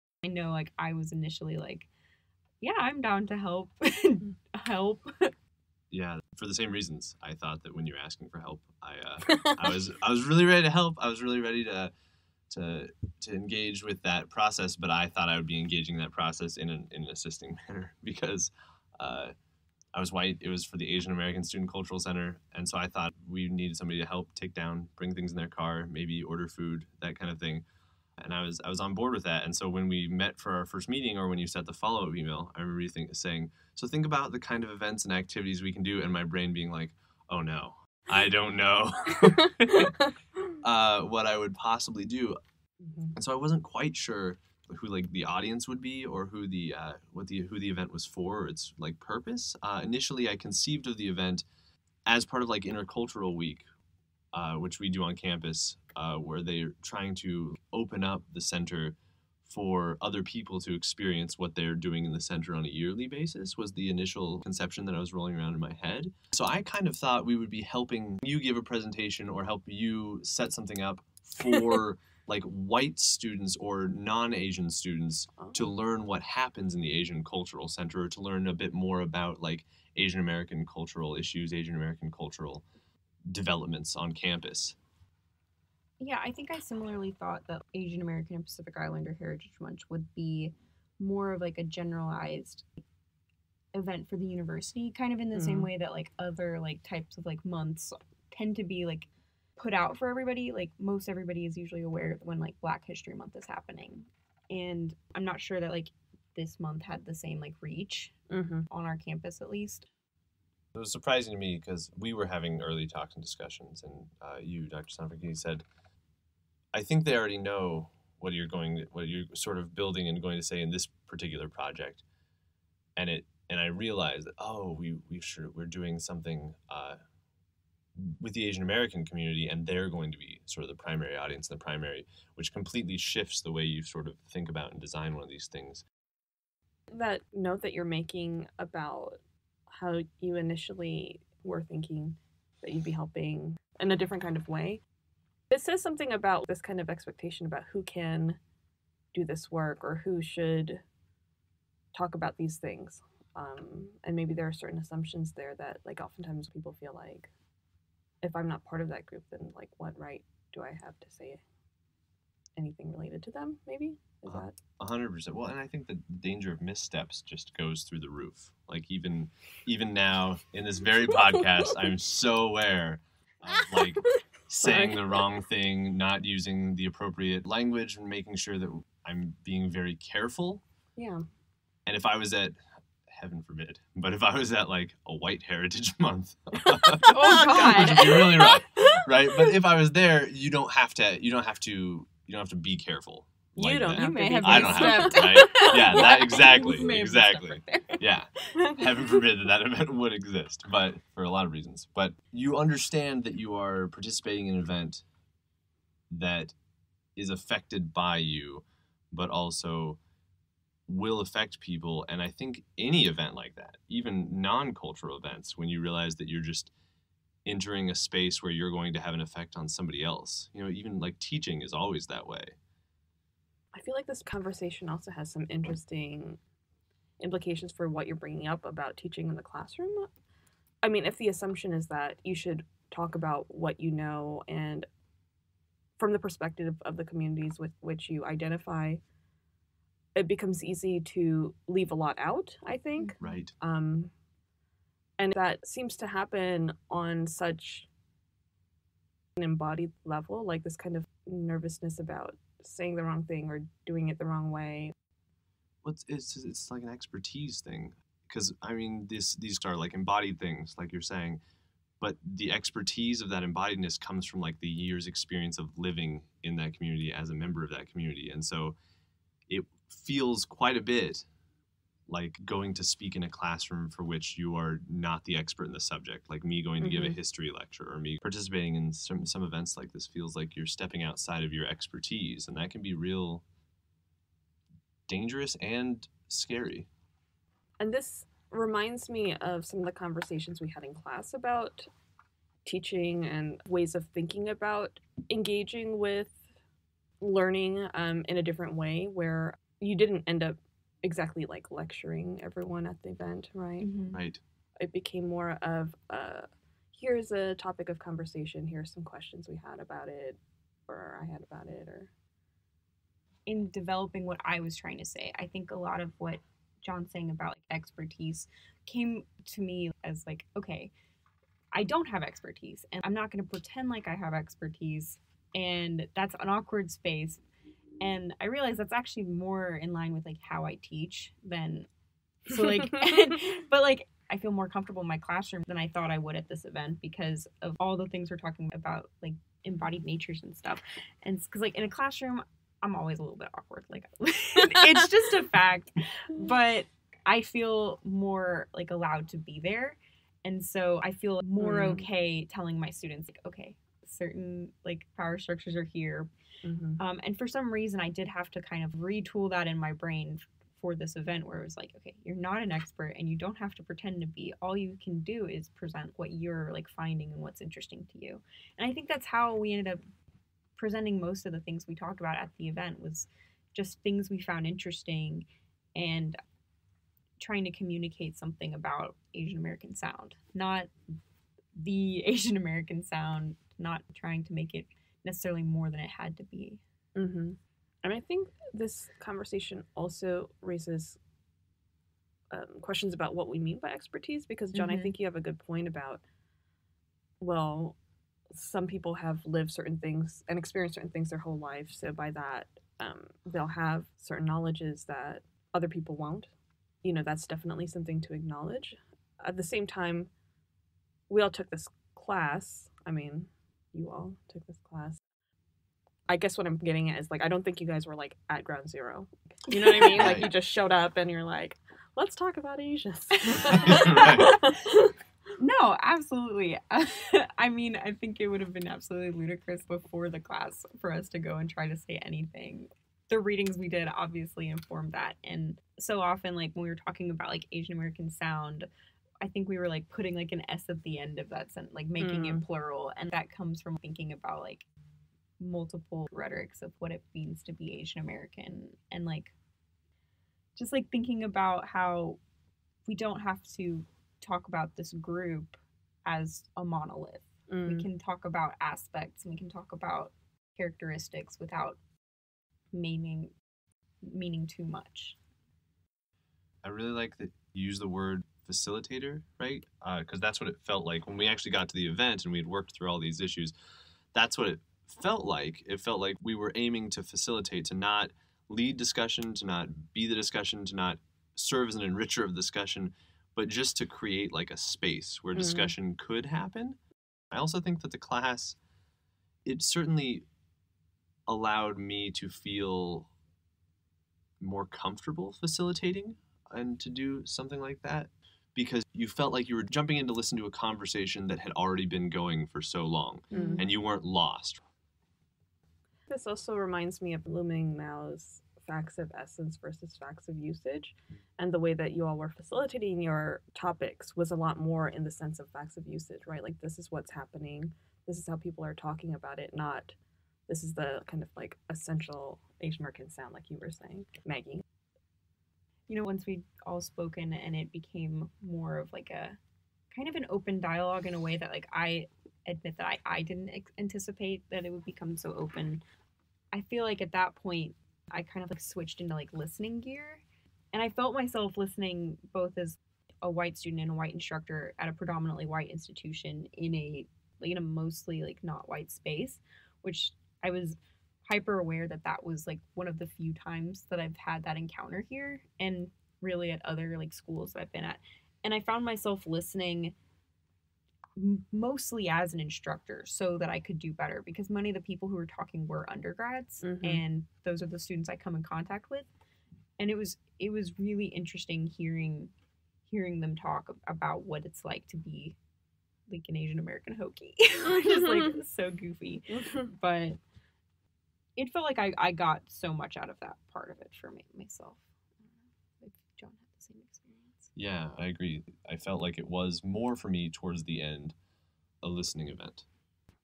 Reflections on Our Positionality, Part 2
Part 3 of 4 from Jennifer Sano-Franchini, Margaret Fernandes, Jonathan Adams, and Michelle Kim's webtext "Sounding Out in a PWI: Circulating Asian American Sound for Institutional Change." Continuation of a conversation among the authors in which they each reflect on their positionalities in relation to the event. This text is published in the 26.1 issue of the Praxis section of Kairos: A Journal of Rhetoric, Technology, and Pedagogy. Kairos is a free, online, peer-reviewed scholarly journal available